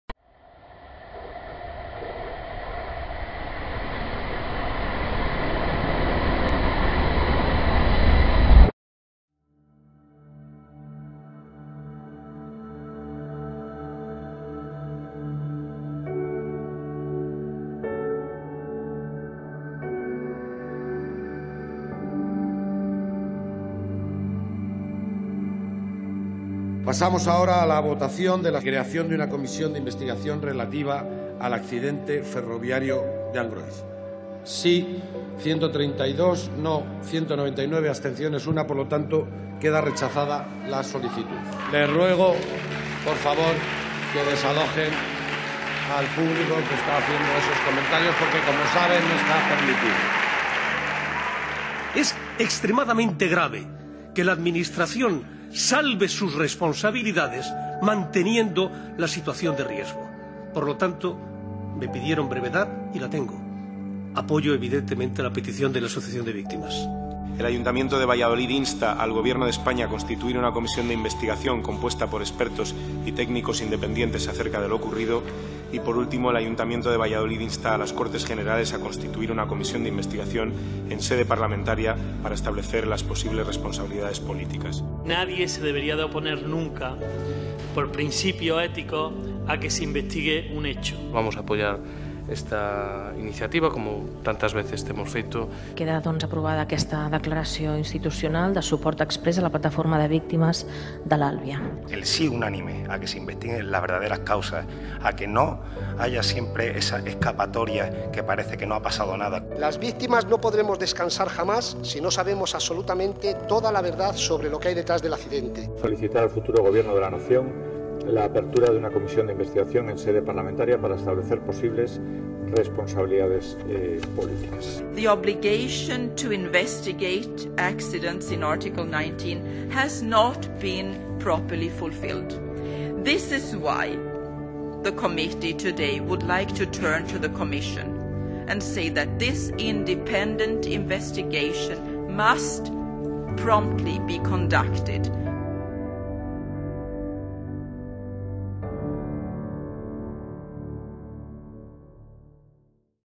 Las víctimas de Angrois piden una investigación
Más de cien ayuntamientos apoyan su solicitud.